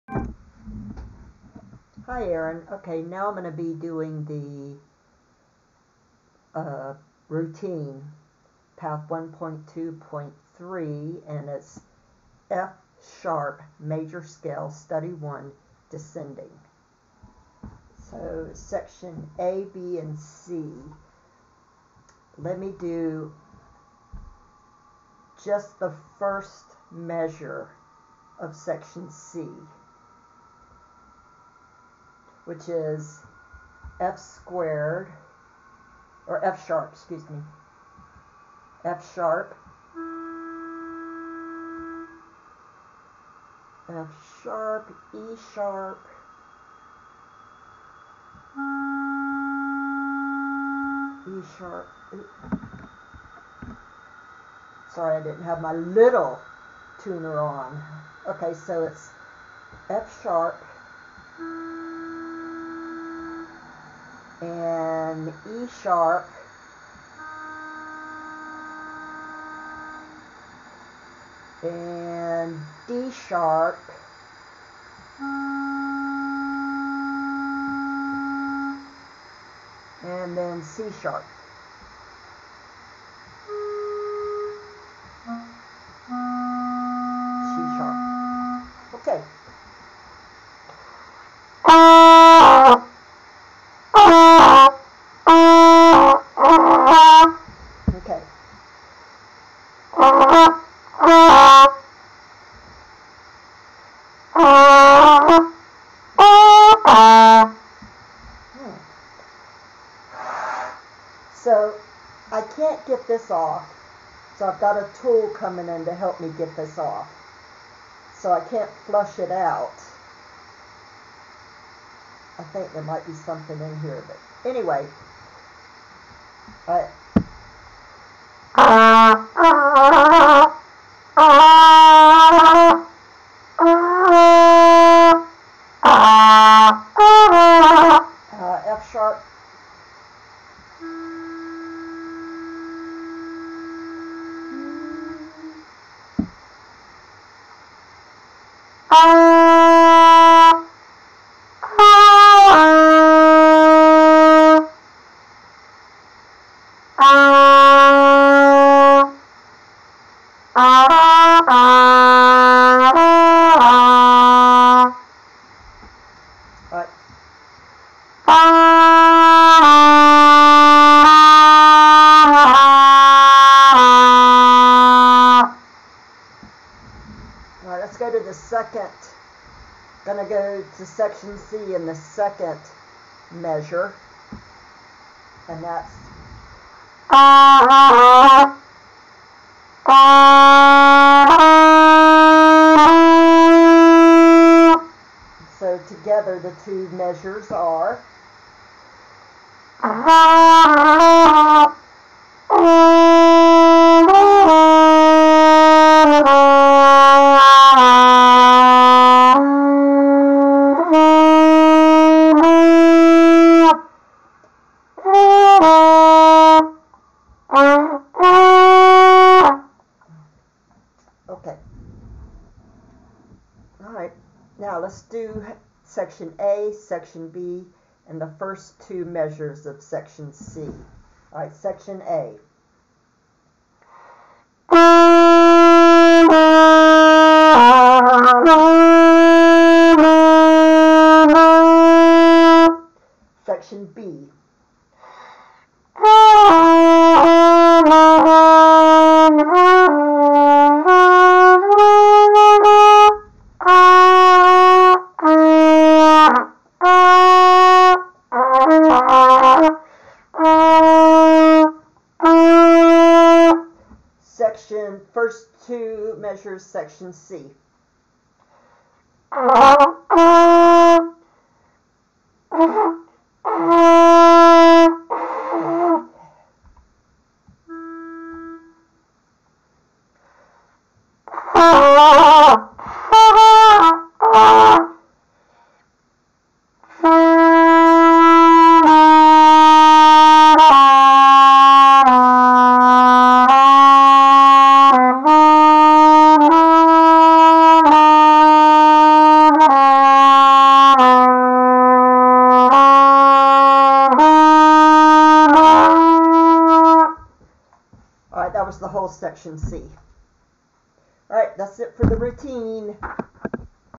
Routine Path 1.2.3 F# Major Scale Study 1 Descending.
Routine Created by Aaron Washington The Black Trumpeter on YouTube he's also my Trumpet Instructor 🎺.